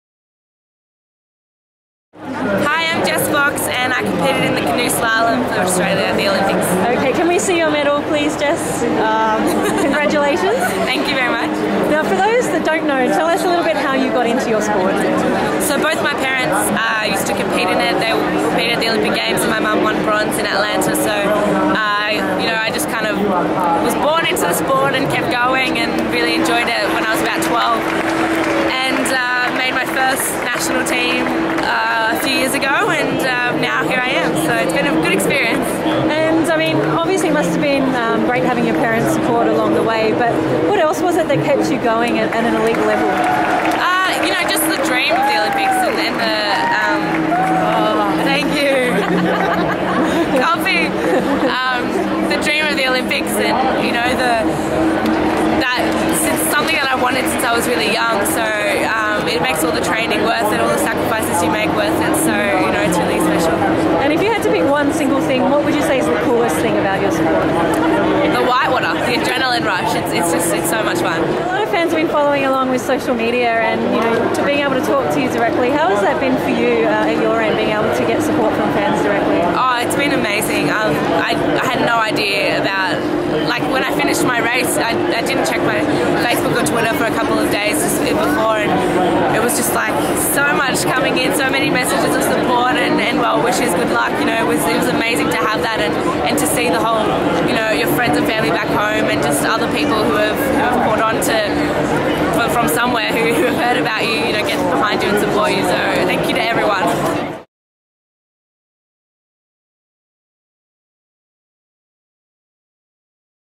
Jessica Fox, Silver Medalist, Canoe Slalom
chats to Jessica Fox (Canoe Slalom) on her return from the London 2012 Olympics.